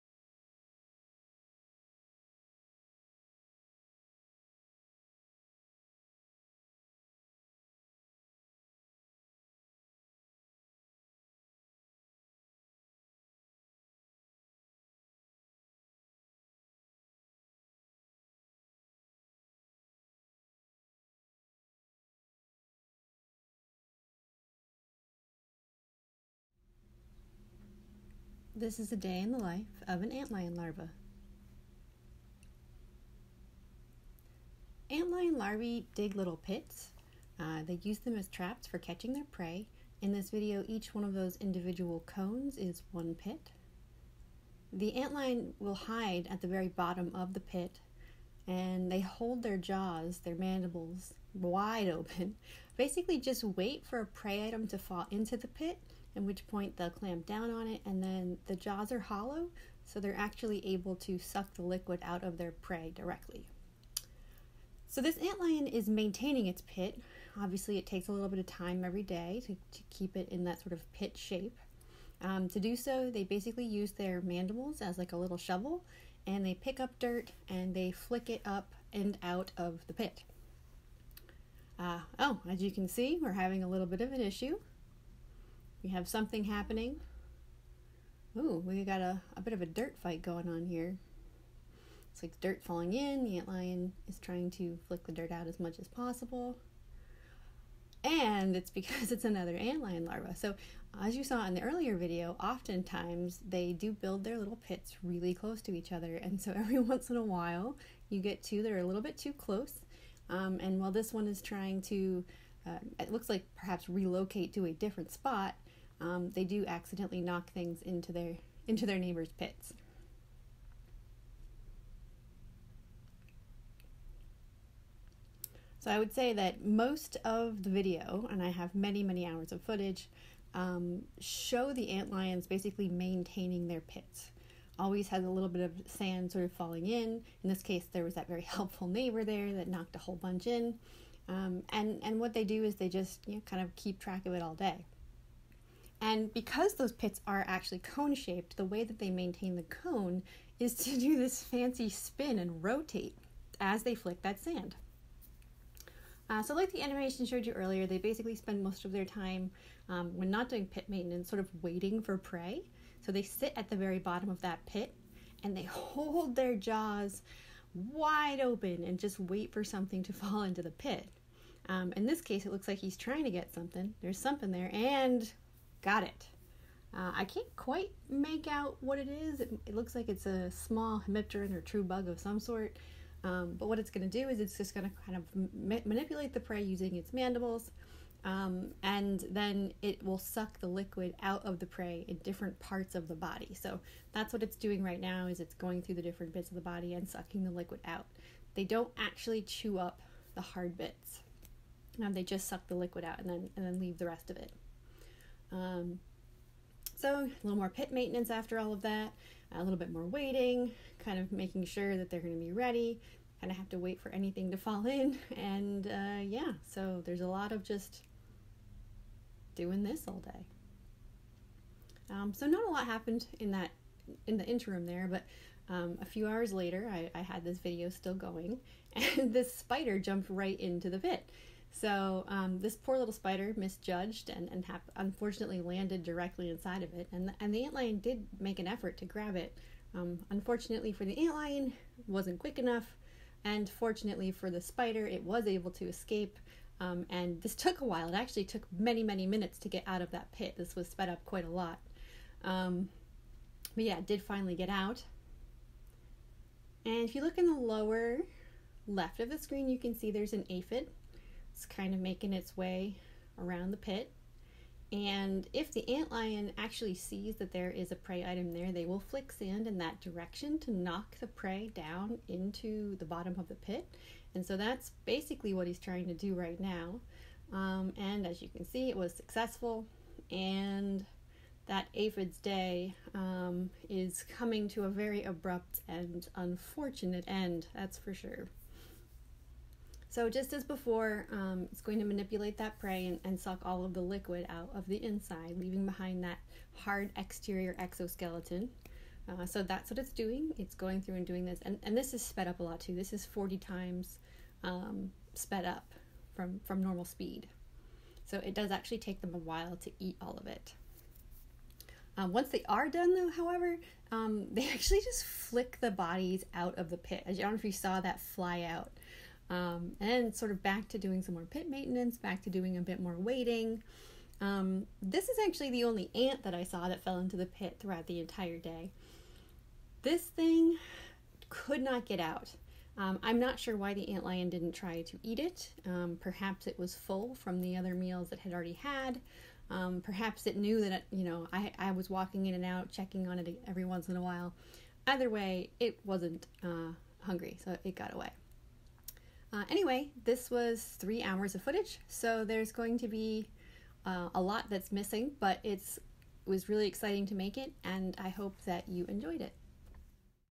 A day in the life of an antlion larva
I took about three hours of video of an antlion (Myrmeleontidae) pit showing the typical day-to-day activities of the larvae of these voracious, trap-setting predators. You'll see pit trap maintenance, a few smalls meals, some neighborly disagreements, and a few surprises. I hope you enjoy it and find it informative. Video was recorded on 7 June 2021 in Northhampton Co., Pennsylvania, USA.

Please note: I mention that the mandibles are hollow - to clarify: there is a space between the mandibles and another set of mouthparts (the maxillae) that forms the tube through which the inside of the prey insect are sucked up by the antlion.

All videos and graphics created by Dr. Amanda Whispell.